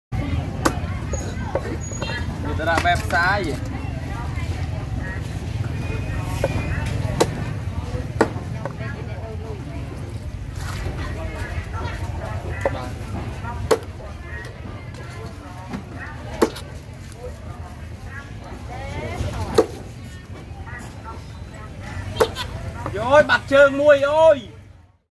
Decapitating Frogs in the Marketplace
This is pretty much a daily activity at local markets. Most of the frogs then get bought up, cleaned, and stuffed, before being grilled over charcoal and sold at roadside stands.